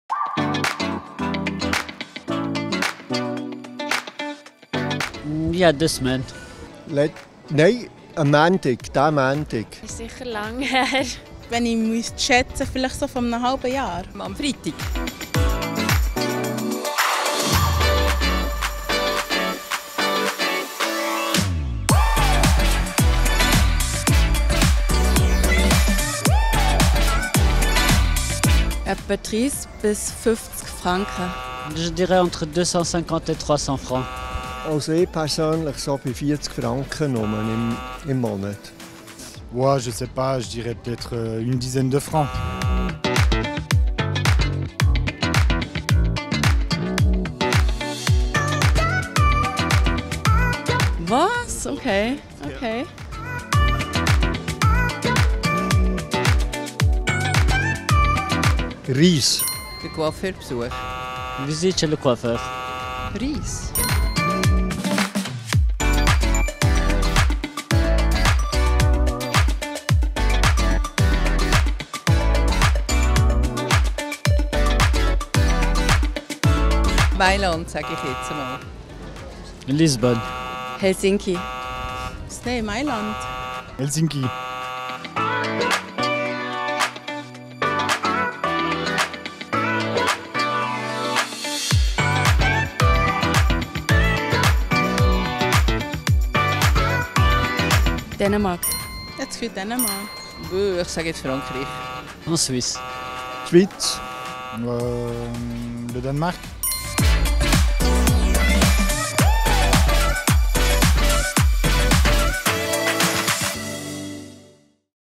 Strassenumfrage zur Preisentwicklung der Post
Die Post hebt ihre Preise für den Briefversand per 1. Januar 2022 leicht an. Doch wie oft verschicken wir eigentlich noch Briefe? Wie viel gibt ein Schweizer Haushalt für Postdienste aus?  Und wie steht die Post in der allgemeinen Preisentwicklung und im Vergleich mit anderen Ländern da?
Wir haben uns dazu auf der Strasse umgehört.